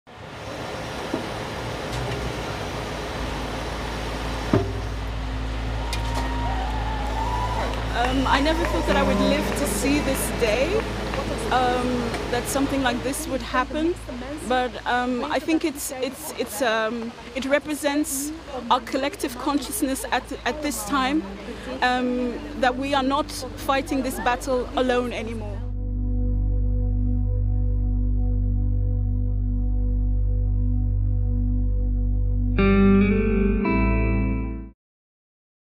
Bust Of Colonialist King Leopold II Removed In Belgium
Snapshot | A bust of colonialist King Leopold II was removed from the city of Ghent in Belgium